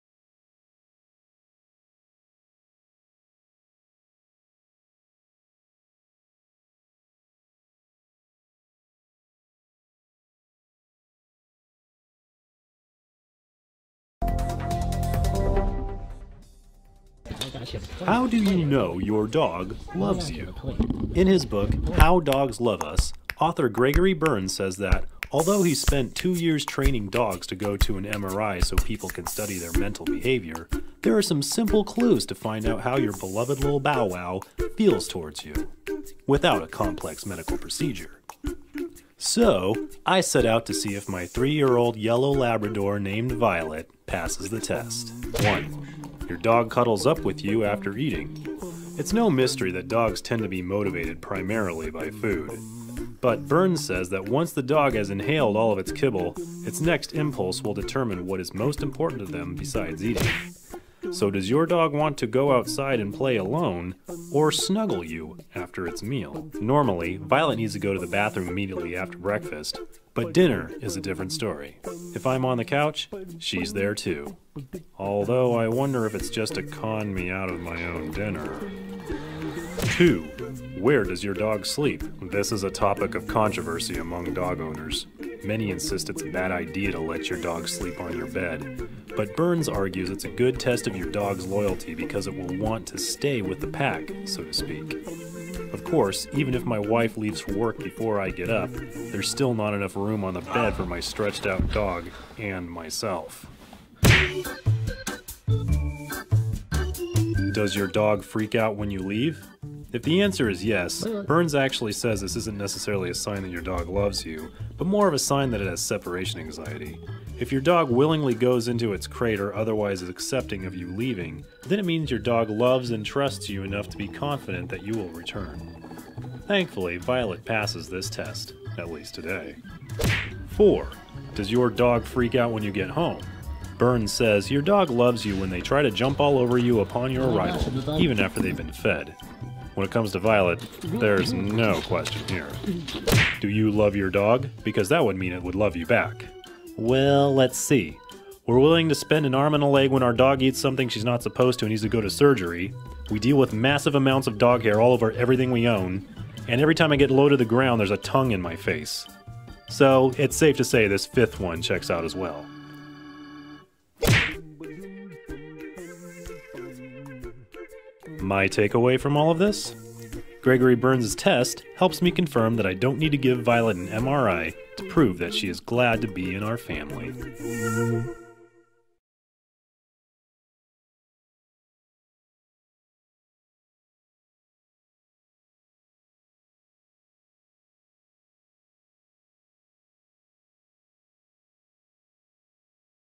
How to Know Your Dog Loves You
Neuroscientist and author Gregory Burns has five ways to test your dog's loyalty to you without using an MRI. WSJ's Billy Higgins puts his three-year-old yellow Labrador to the test. Give it a try and post your experiences below!

Click at this link to get free Dog Healthcare Books & Guides